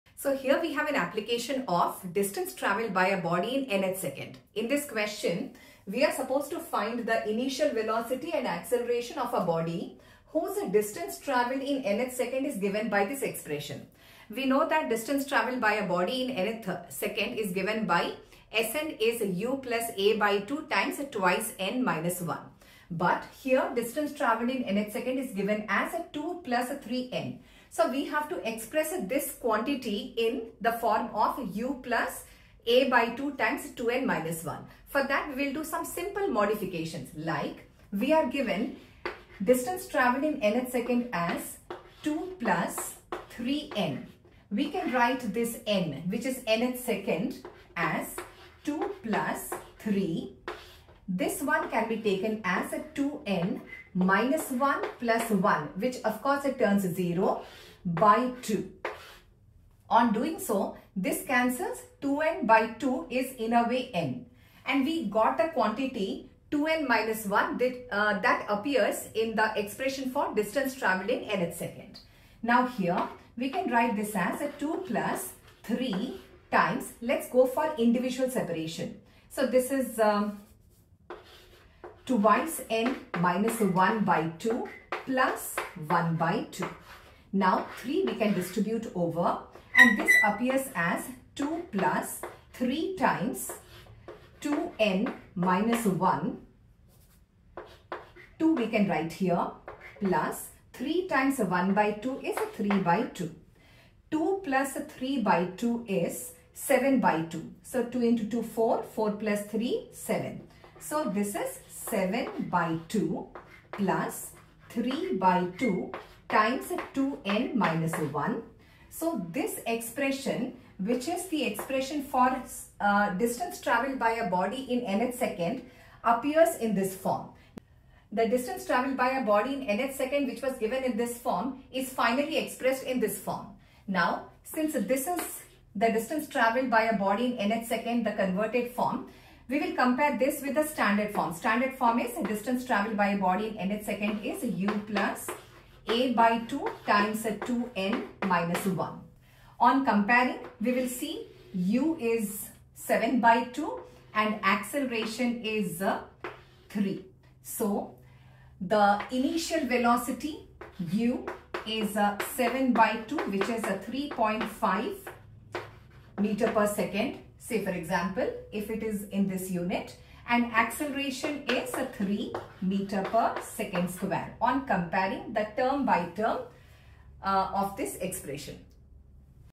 Kinematics I Pearson IIT foundation class 9 Level-2 question's solution.
@PW-Foundation @PhysicsWallah Find initial velocity and accelaration of a body whose distance travelled in n-th second is given by the expression Sn = 2+3n.